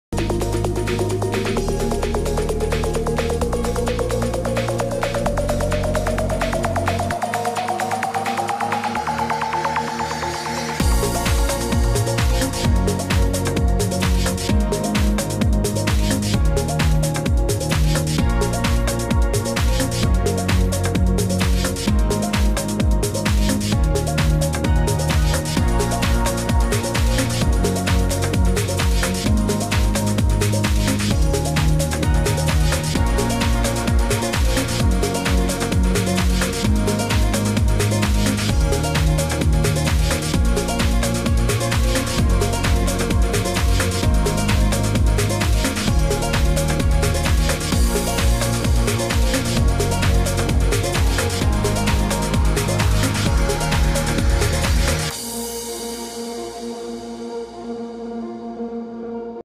Quando il siluro è davvero tanto grande...
Epico combattimento di Lorenzo @redangler1513 con un enorme pesce siluro allamato sul fiume Tevere